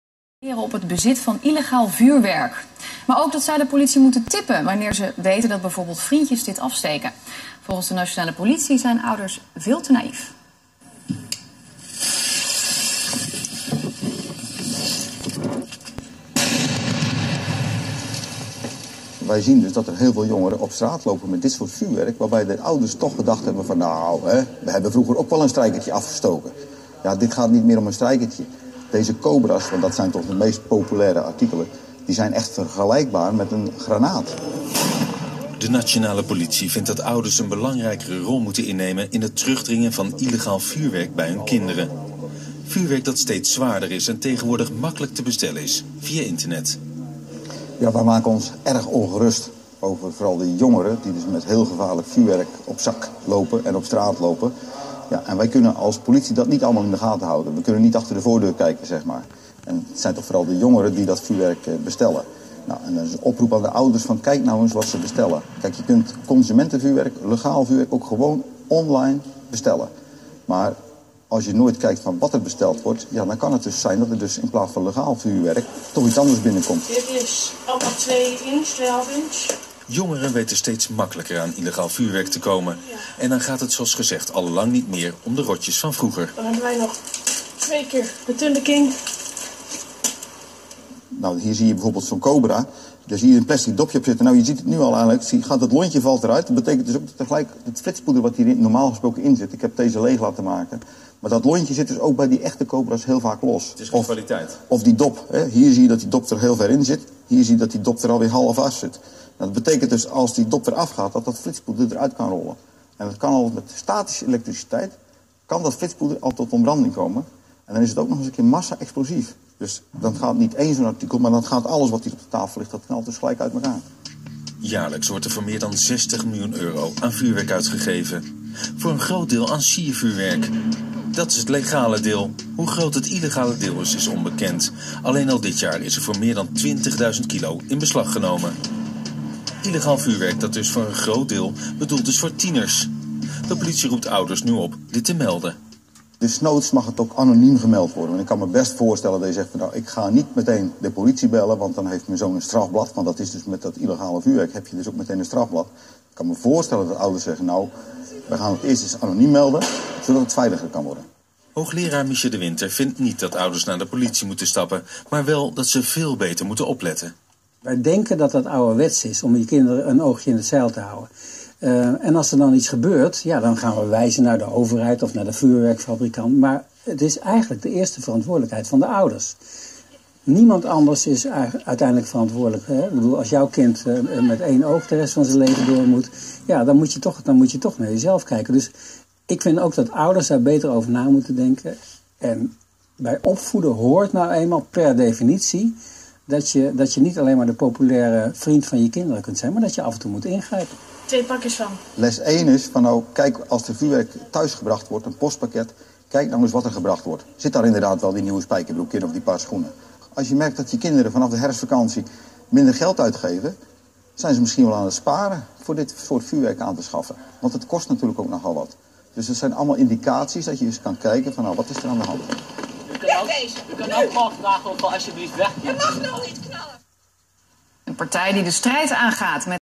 Politie #'Ouders naïef over illegaal vuurwerk'
illegaal vuurwerk'. Cobra's, vlinders en# nitraatbommen: het zijn klinkende namen van illegaal vuurwerk. Aantrekkelijk voor kinderen, maar wel de reden dat elke jaarwisseling jonge slachtoffers in het ziekenhuis belanden. De politie roept nu ouders op verantwoordelijkheid te nemen voor het vuurwerkbezit van hun kinderen.